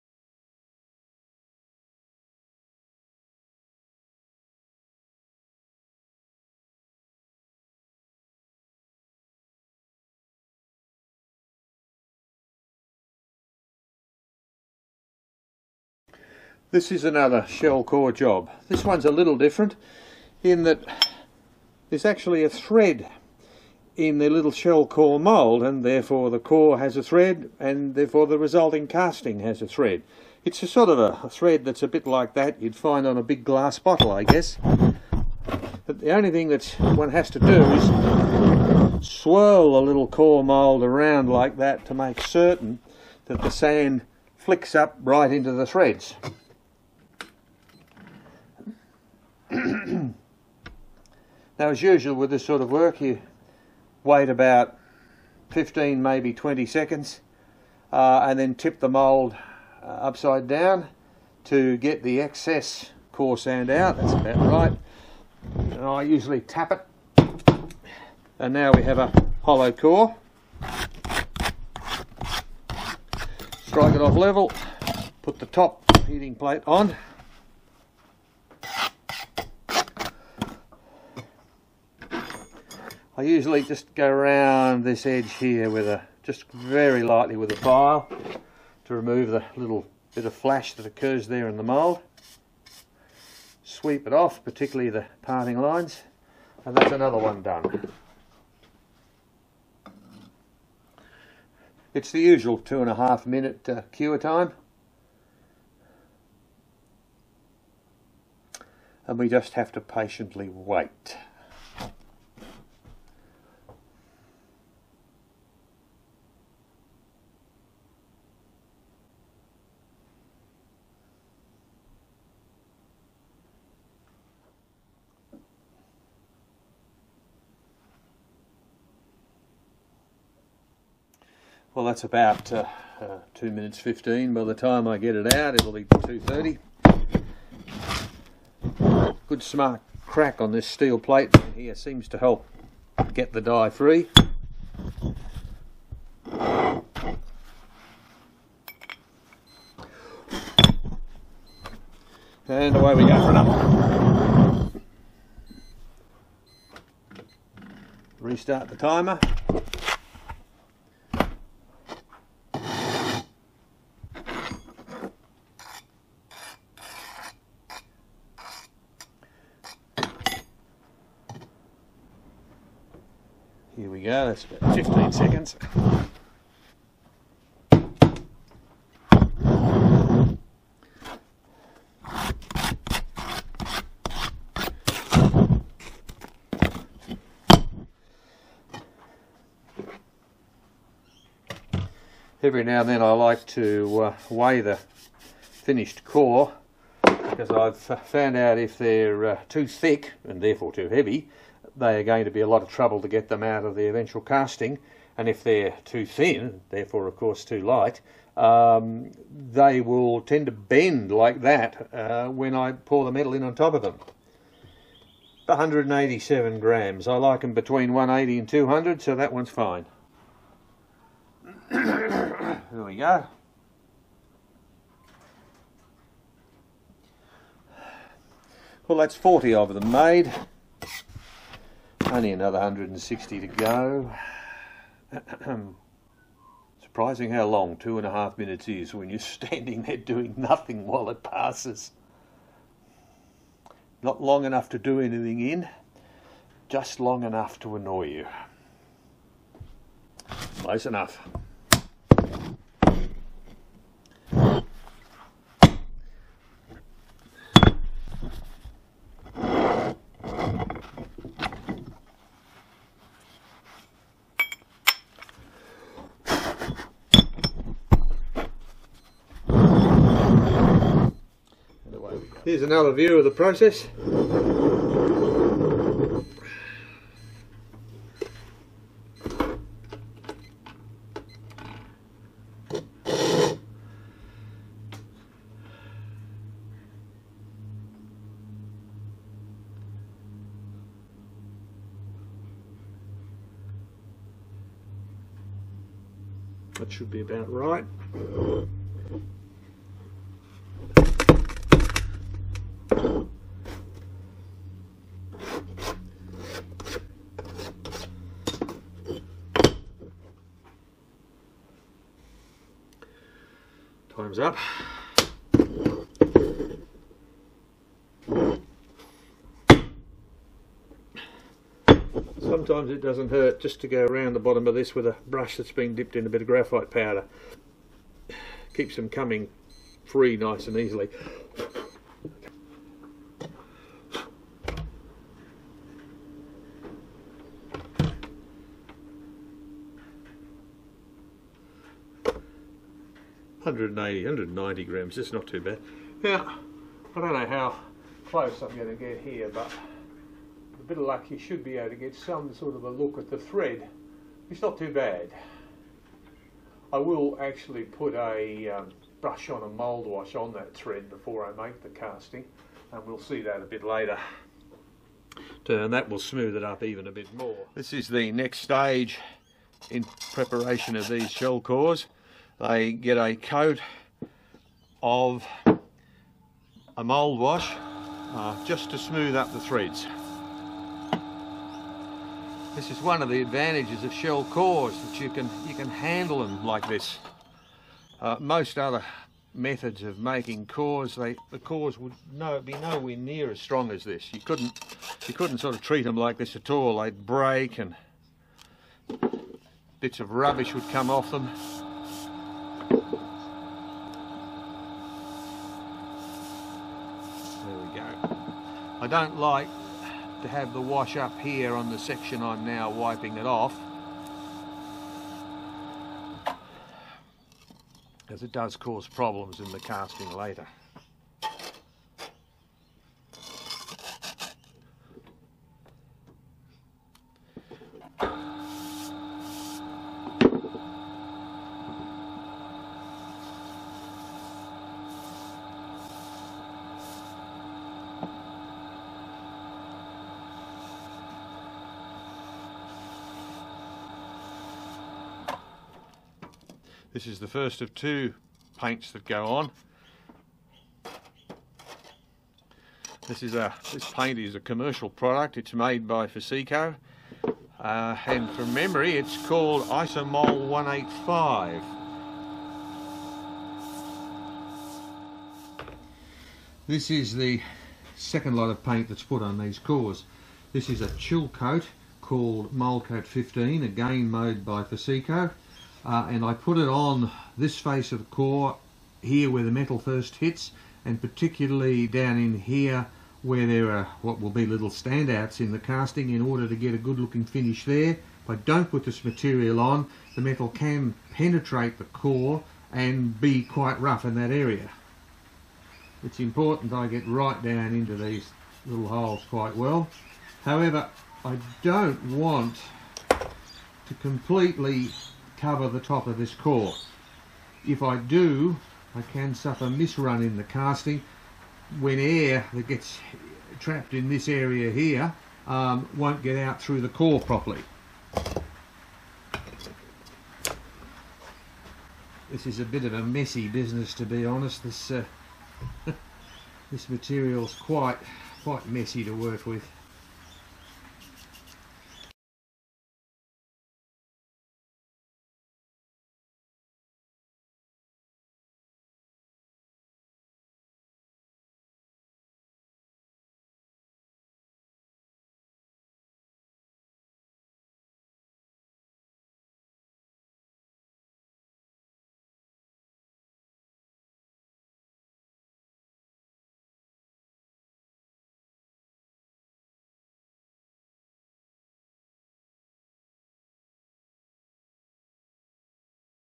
Casting Runway Landing Lights Part 1 The threaded shell sand cores
A large runway at a busy airport can easily have over 1000 lights. Some at each end, some down each side and even some set in the runway surface itself. The lights made here in part 1 and 2s sit on short stands at the end of the runway. They were originally made from a filled plastic but the strong UV light in Australia shortens the life of the plastic so these are made from aluminium and are powder coated to produce the required yellow finish.